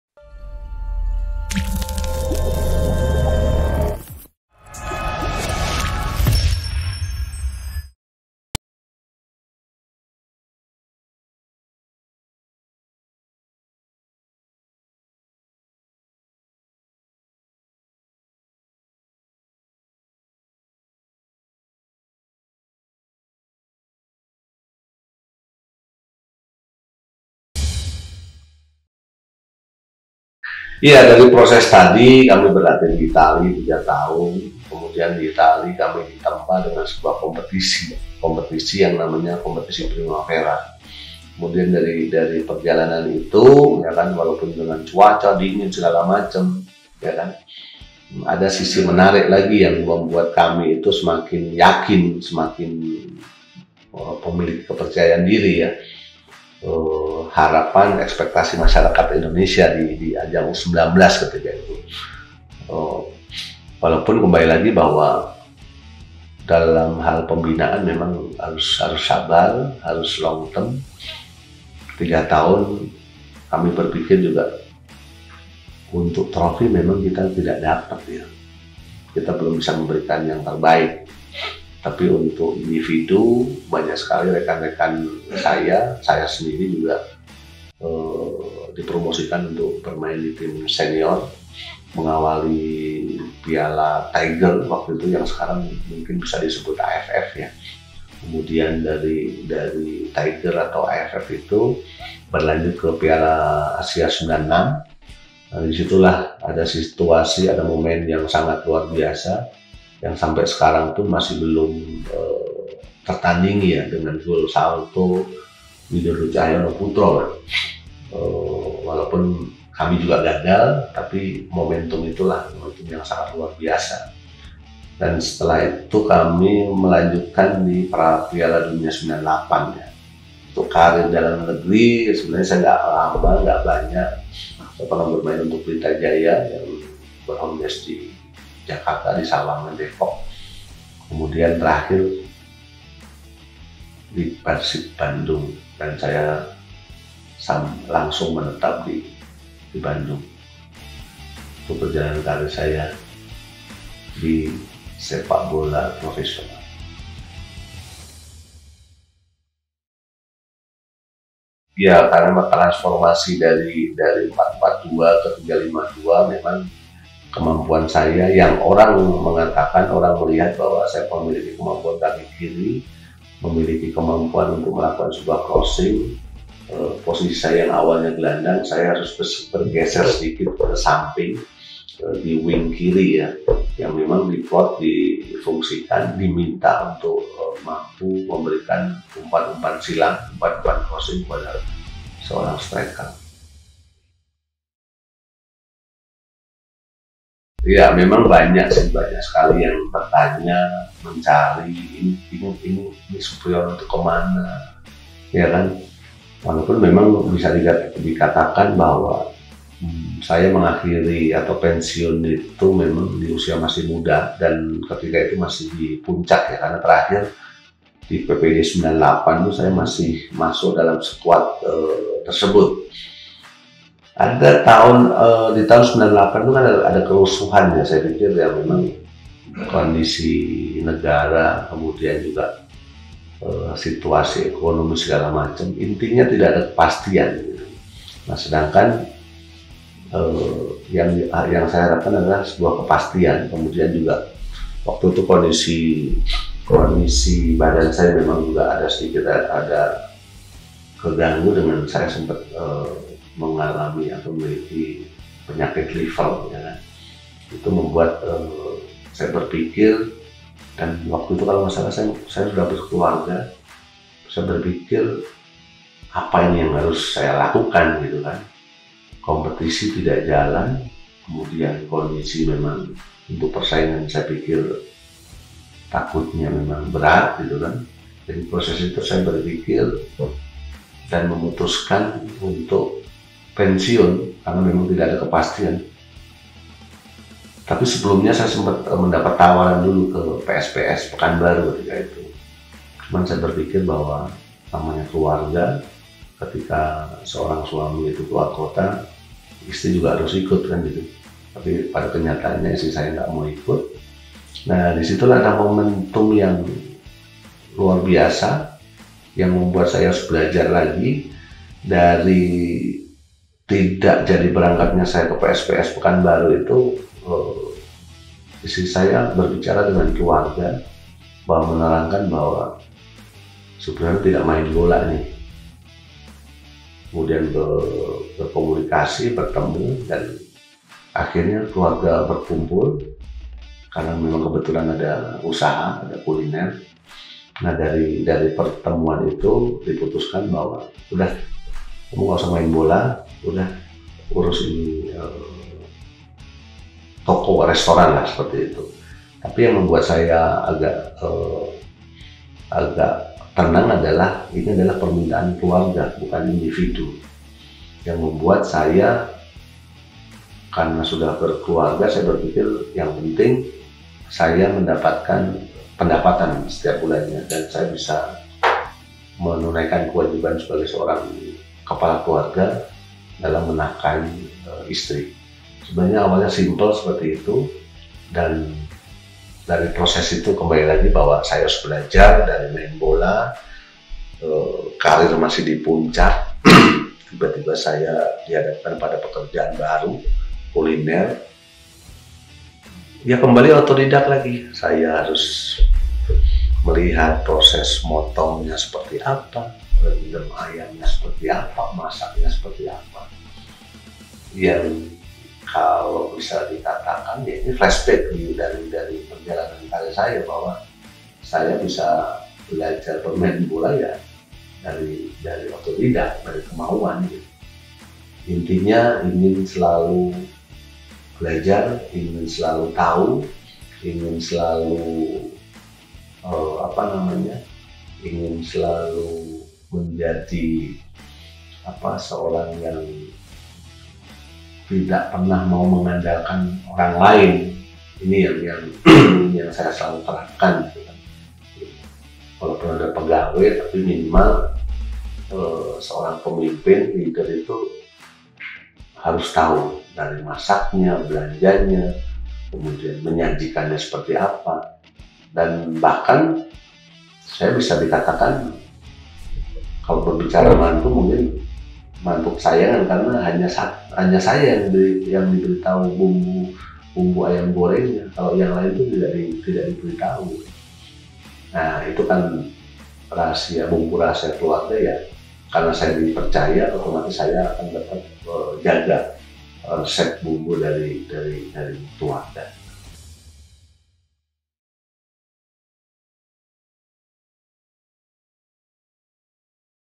Perjalanan karir Supriyono Prima part 7 dan 8
Coach Supriyono merupakan salah satu mantan pemain primavera nasional bersama Kurniawan, Bima Sakti dan Kurnia Sandi pada tahun 90an. Sempat membela PERSIB sekarang setelah pensiun Coach Supriyono mendirikan sekolah sepak bola bernama Bintang Primavera. Tidak melupakan kesuksesannya bersama tim primavera nasional, Coach Supriyono ingin mengembangkan bakat muda pemain bola.